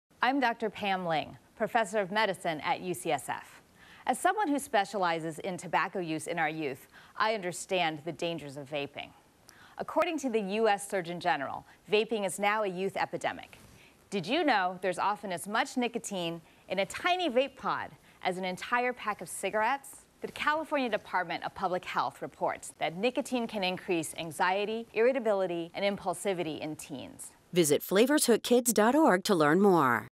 Pam Ling's warning on vaping and e-cigarettes. from KGO San Francisco
Here's Dr. Pam Ling doing a quick PSA discussing the dangers of vaping and e-cigarettes.
Rock. Star.